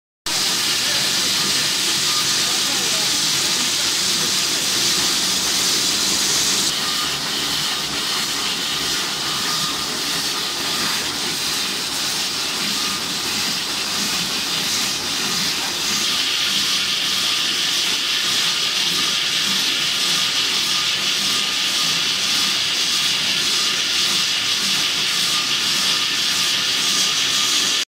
Adhesive BOPP packaging tape jumbo roll two color flexo printing printer machine 500mm
This is BOPP jumbo roll tape flexo printing machine.
It print two brand color on transparent BOPP tape roll.